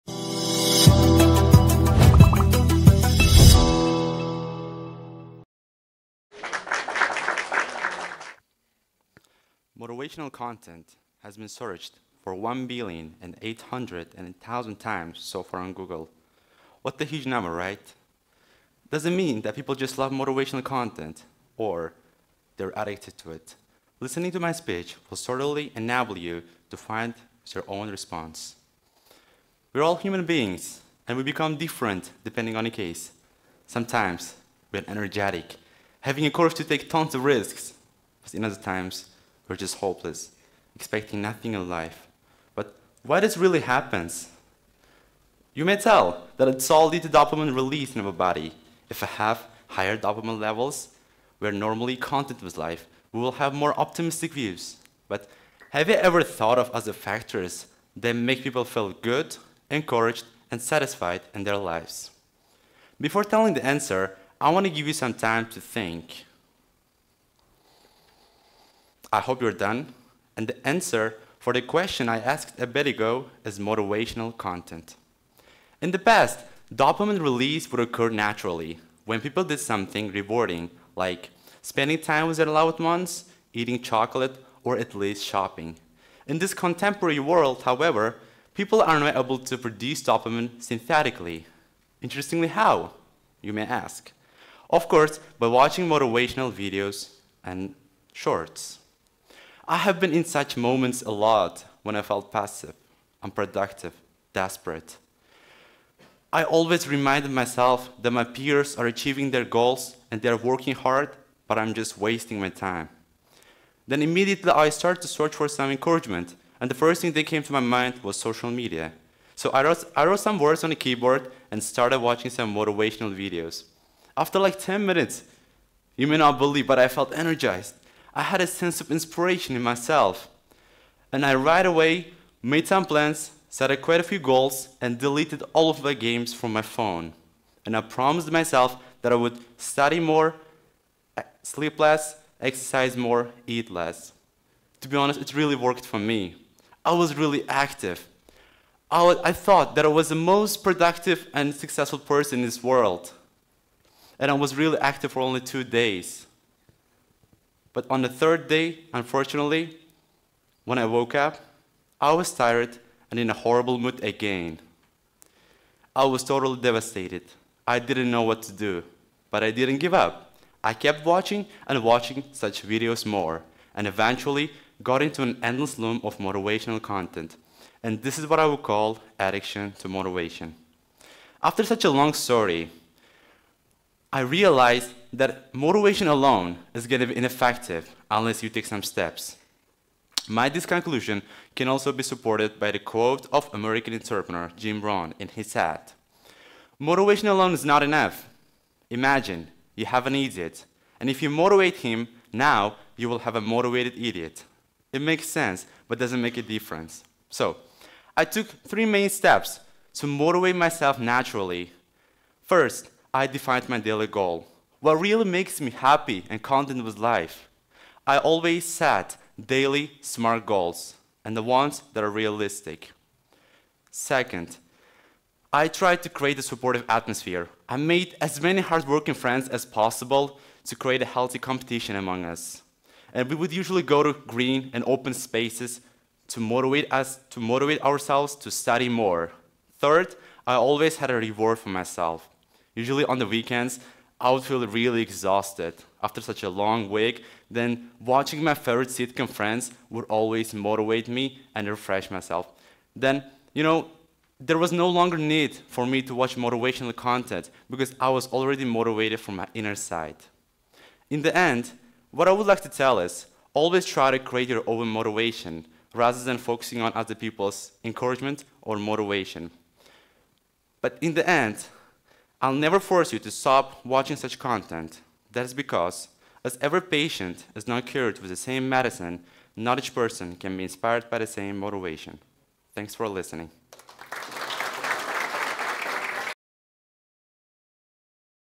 Addiction to motivation | Ozodbek Eshboboyev | TEDxYouth@CICTashkent
Young people often become victims of the illusion of motivational content on social media. In pursuit of a momentary synthetic energy boost, they get into the endless loom of motivational videos and shorts and eventually suffer from burnout or self-deception. This video addresses the question of why do people still watch inspirational videos and how to find inner motivation. An inspiring teenager with a great interest in writing. Organizer of different events and tournaments at school. Have also participated and won in a number of National Olympiads This talk was given at a TEDx event using the TED conference format but independently organized by a local community.